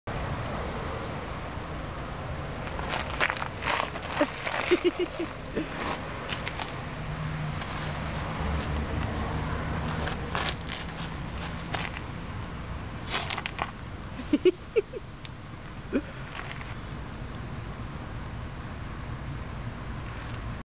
Luke vs. an ice cream cone
Luke tastes his first very own ice cream cone